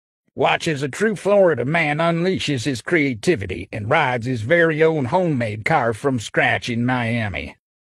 Florida Man drives homemade "car" !!
Watch this wild and wacky adventure as Florida Man crafts his very own homemade car from scratch! Follow his incredible journey from garage to road, showcasing his unique engineering skills and boundless creativity. Buckle up for a ride like no other and get ready to witness the ultimate DIY car project in action. 🚗